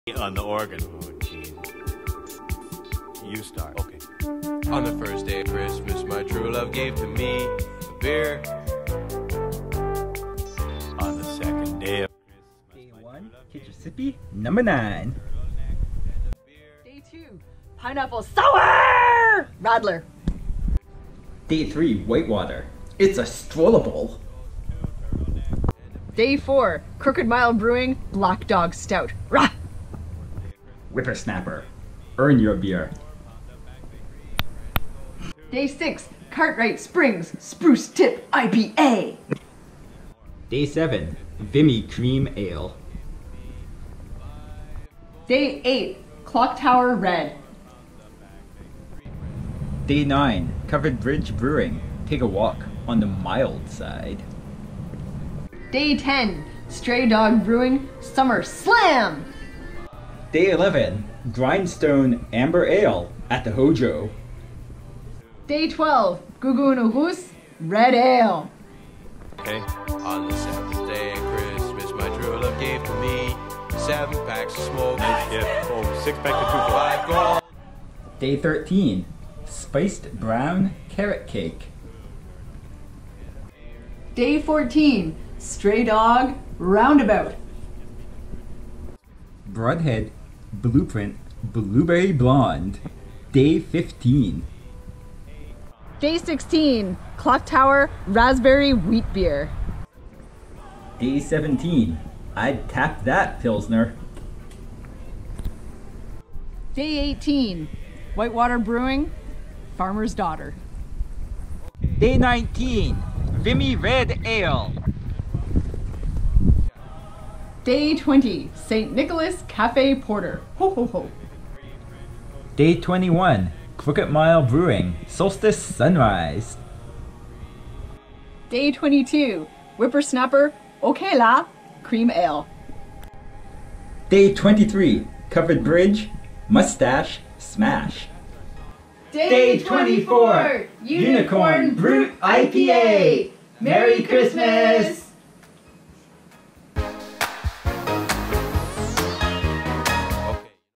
Nita Brewing Craft Holiday Beer Calendar 2018
From blonde, to cucumber, to stout, to IPA, Tim and I drank 24 beers in 24 days, a different microbrew each day.